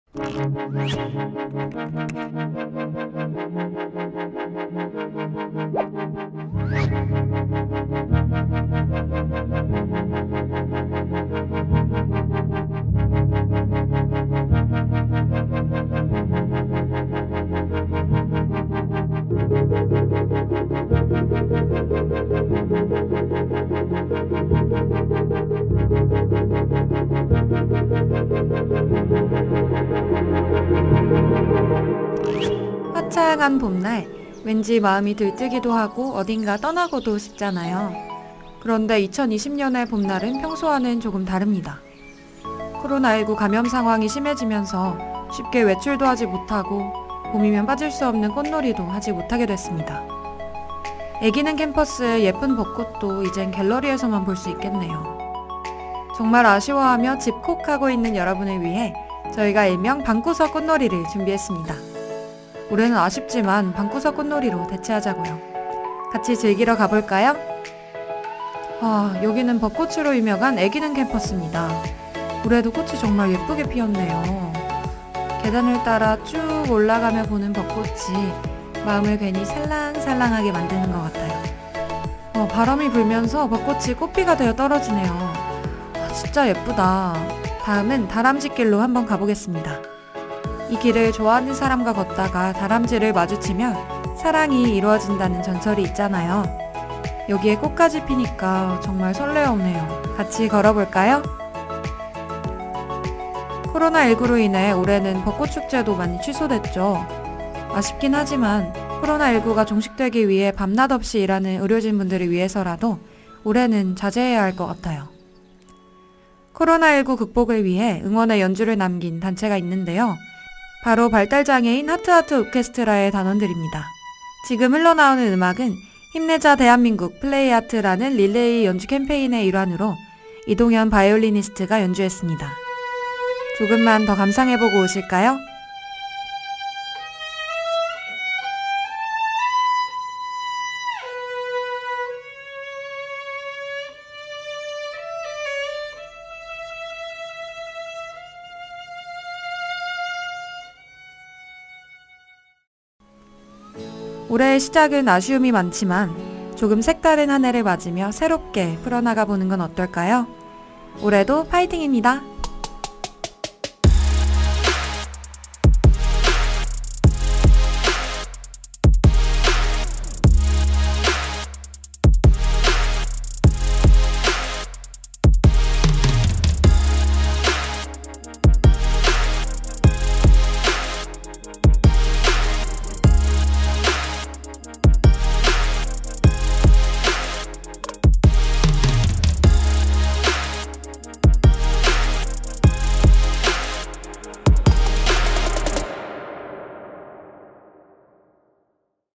[고려대학교 Korea University] 잃어버린 당신의 봄을 위해 - 방구석 꽃놀이
코로나가 앗아간
당신의 2020년, 봄

그저 쉽게는 위로할 수 없단 걸 알기에,
고민하다가 2020년 캠퍼스 꽃 풍경을 준비해보았습니다

멀리서나마 각자의 자리를 지켜내며
부디 행복한 봄을 누리시길 바랍니다

DJ. 미디어학부 19학번 황지원